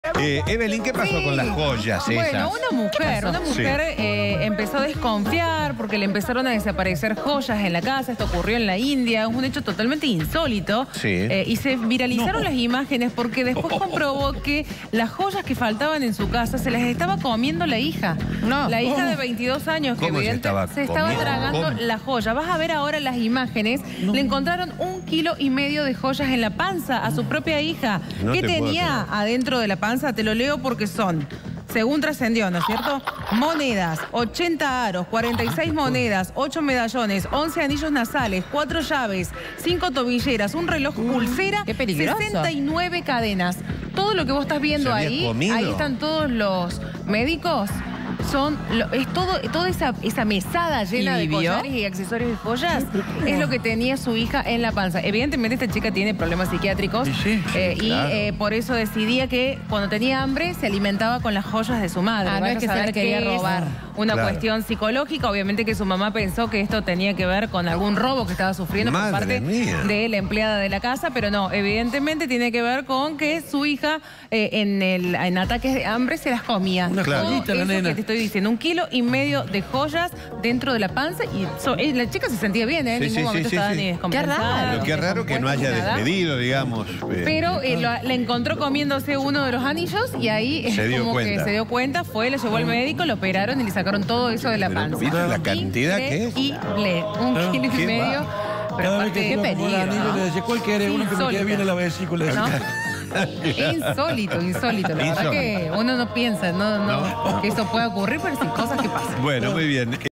Le faltaban las joyas, y las encontró donde menos se esperaba
Los hechos ocurrieron en un pueblo de la India. Un hombre descubrió que estaban faltando una gran cantidad de joyas de su casa, por lo que decidió investigar, y se dio cuenta de que su hija se las estaba comiendo.
Aparentemente, la hija tenía ataques de hambre, por lo que empezó a comer las joyas familiares. Una vez que su padre la descubrió, decidió enviarla a un hospital cercano, donde la operaron, y le sacaron una inmensa cantidad de alhajas.